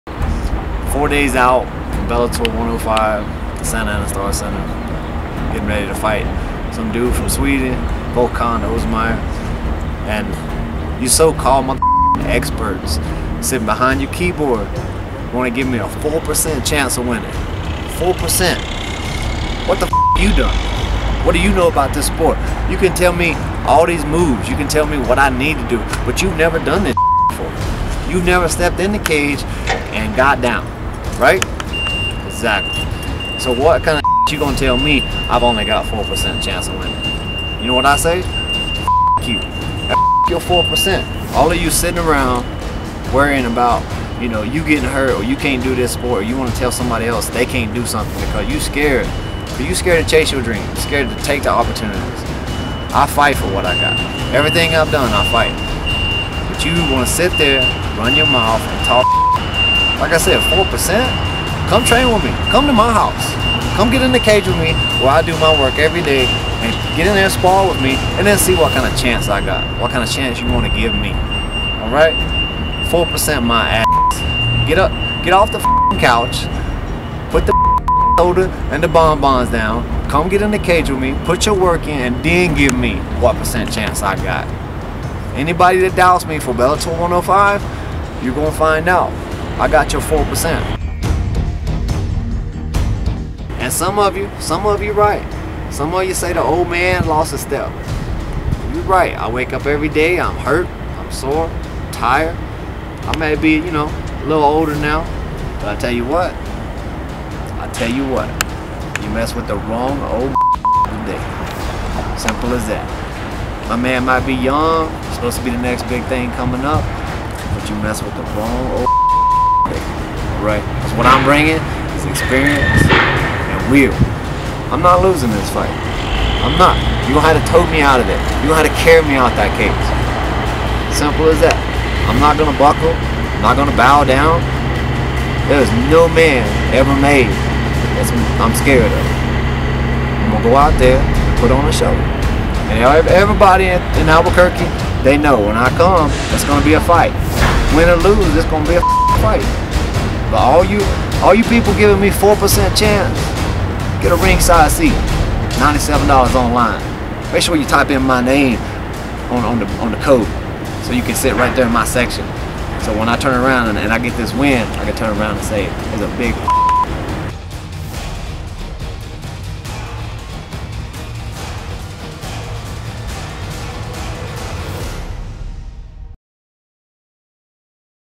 F*** You and Your 4%
Josh Lanier gets ready for Bellator 105 coming to the Santa Ana Star Center on Friday October 25, 2013.